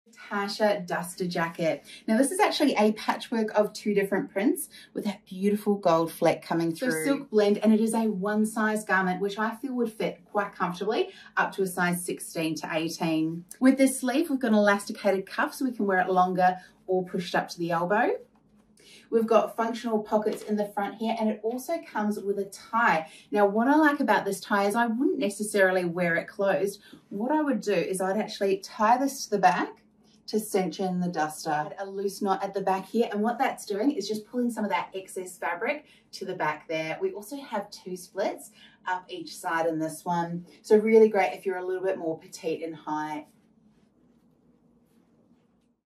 Tasha Duster
The Tasha Duster jacket is perfect with a thoughtful sleeve design and functional pockets.